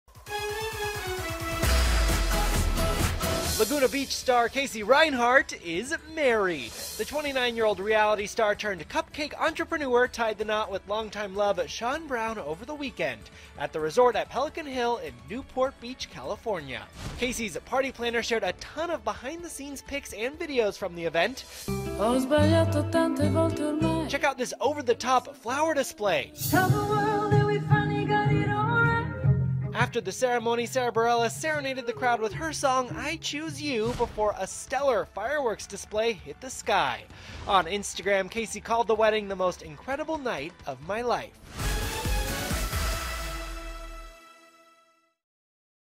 'Laguna Beach' Star Casey Reinhardt Ties the Knot in Lavish Newport Beach Wedding
The cupcake entrepreneur married her longtime love Sean Brown.